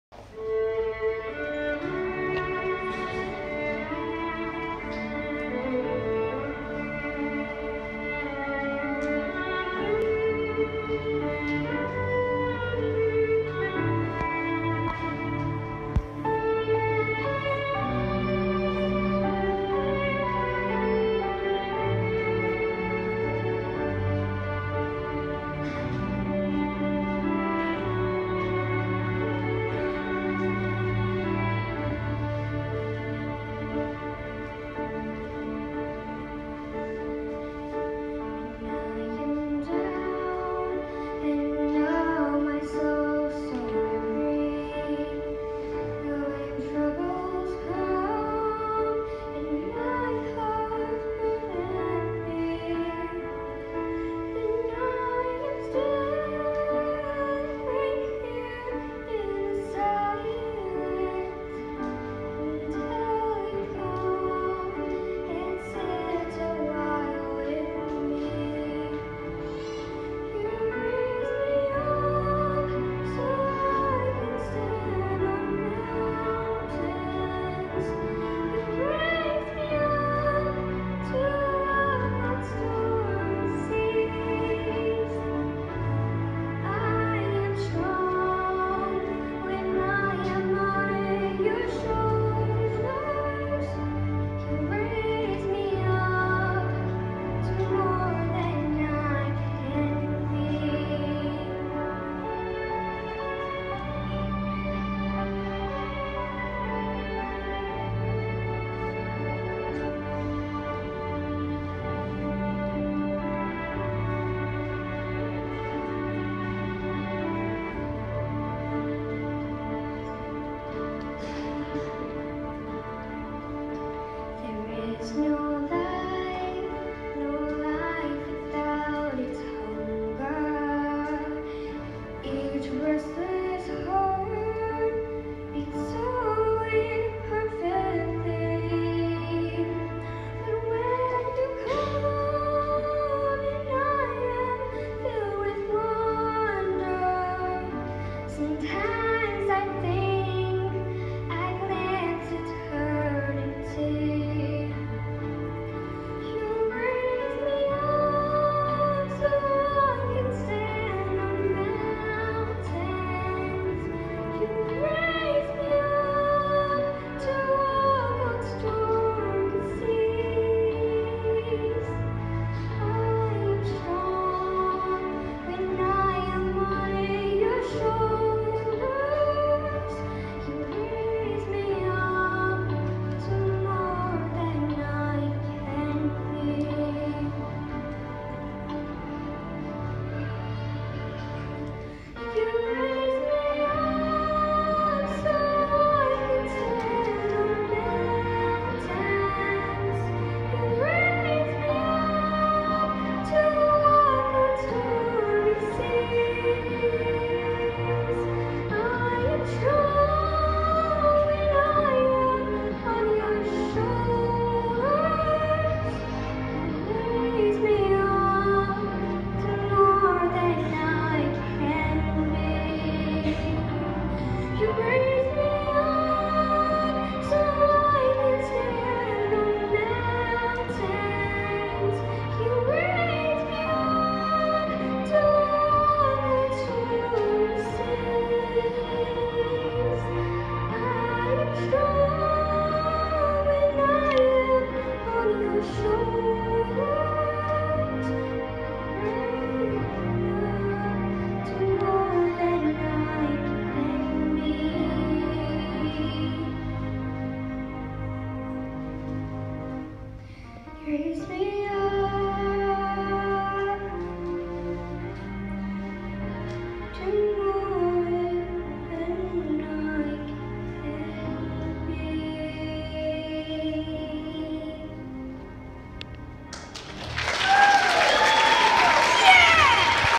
Jaedyn Swails
Jaedyn singing for her school talent show.